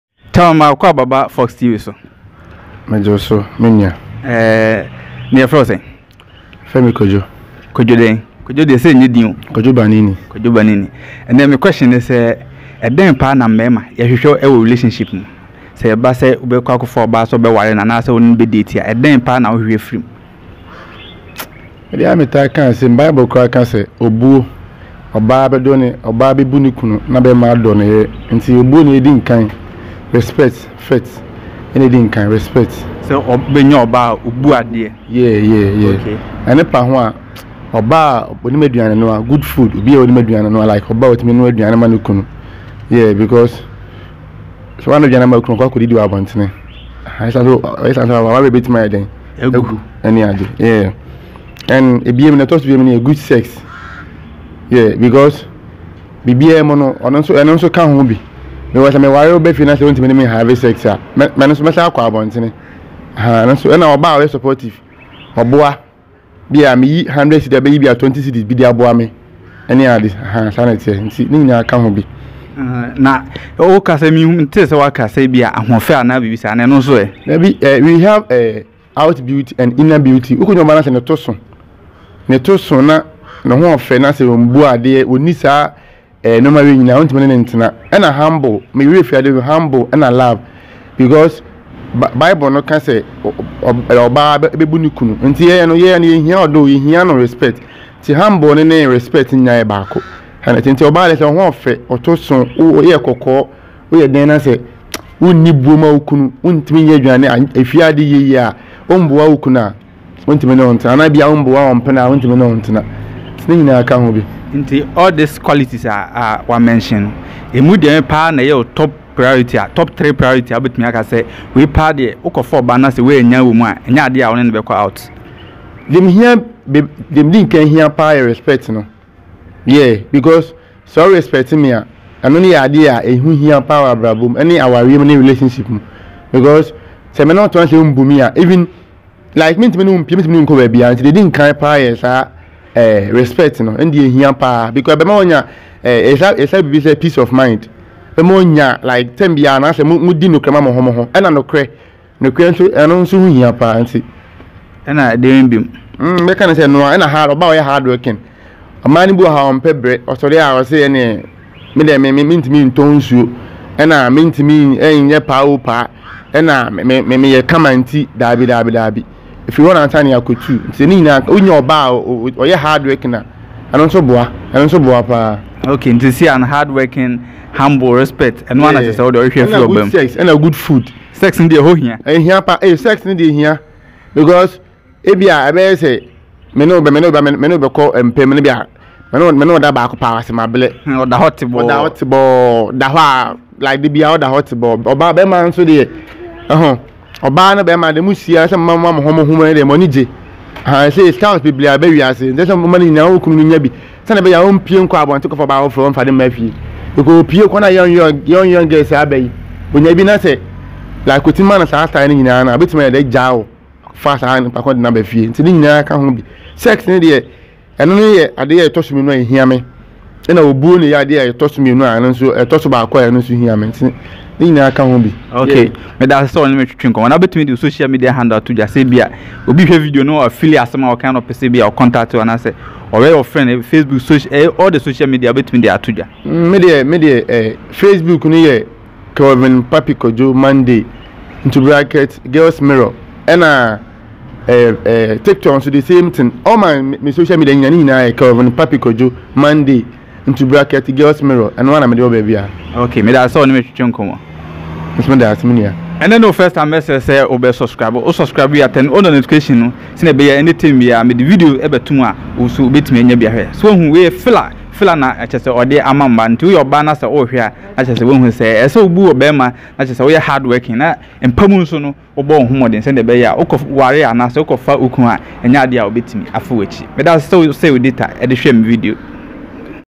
What Do Guys Want In A Relationship | What Men Want | Things Men Want
In this video, I'm on the street of Kwame Nkrumah University Of Science and Technology Campus ( KNUST ) Kumasi asking random guys what at all do men want in a relationship! Don't forget to share this video If you like it, also let me know in the comment below some questions you'd like me to ask for street interviews in the next video.